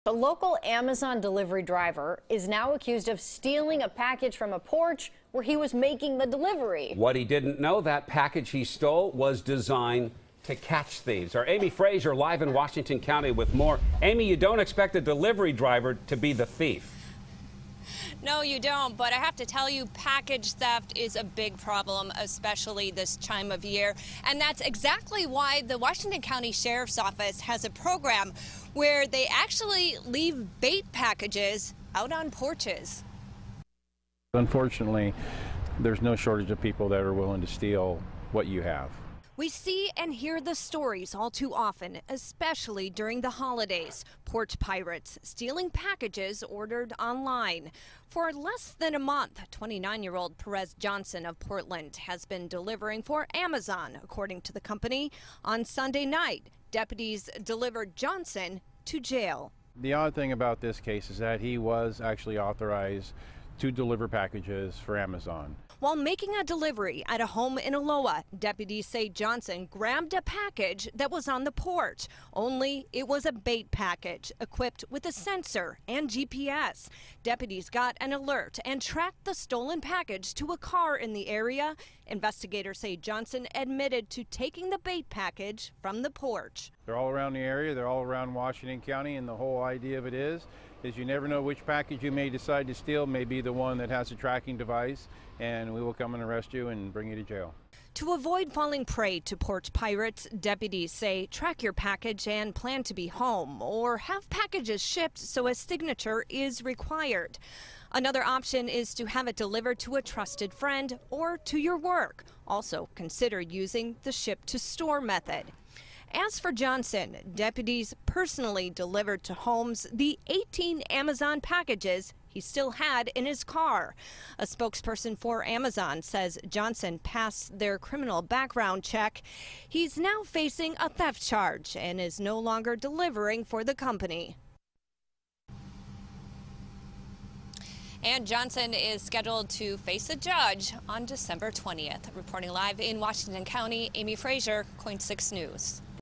Amazon delivery driver arrested after taking the bait
Perez Johnson, 29, of Portland was arrested for 2nd-degree theft and booked into the Washington County Jail.